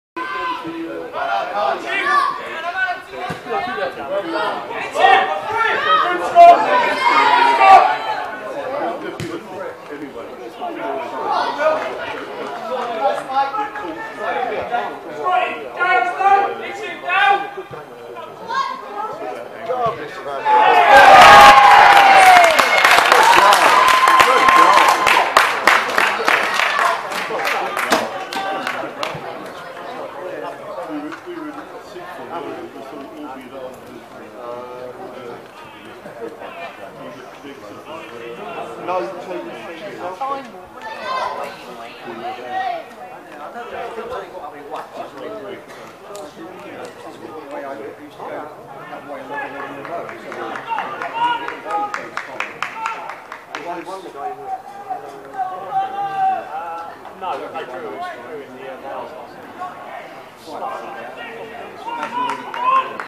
May & Baker 1-3 Burnham Ramblers. Thurlow Nunn Eastern Counties League Div 1 South. Wed28Aug2019
Despite a late rally from M&B, an energetic display from Ramblers ensures they deservedly take the points away from Aveley's Parkside